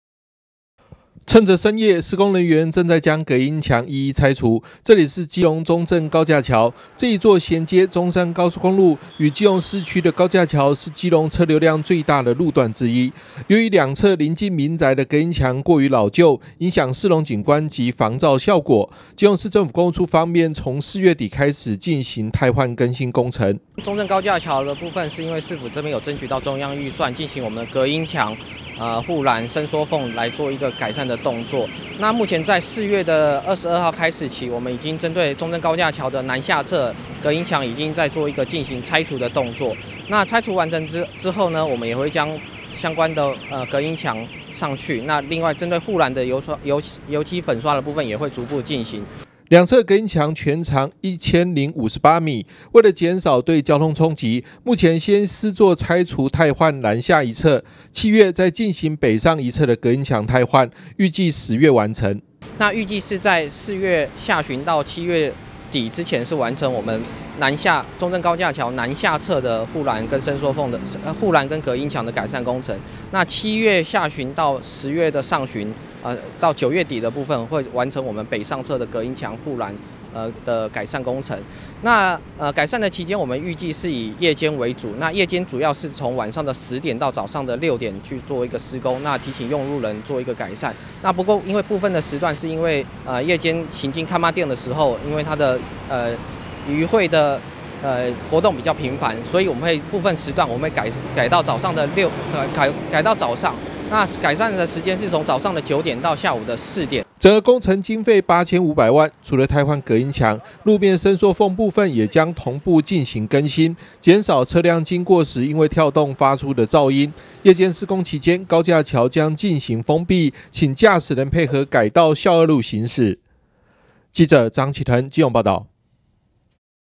交通 資訊|更新 施工|國道 市區|隔音 牆|駕駛人 開車|中嘉新聞 2021|工程持續到十月 中正高架橋更換隔音橋伸縮縫 駕駛人請注意
高架橋隔音牆太過老舊拆除更新夜間封路施工駕駛人請改道

（中嘉吉隆新聞記者張啟騰/基隆報導）銜接中山高速公路與基隆市區的中正高架橋是基隆市車流量最大的路段之一，由於鄰近民宅的隔音牆過於老舊，基隆市政府從四月底開始進行兩側隔音牆汰換與路面伸縮縫更新工程，原則採夜間施工，工期將持續到今年十月，請來往用路人特別注意。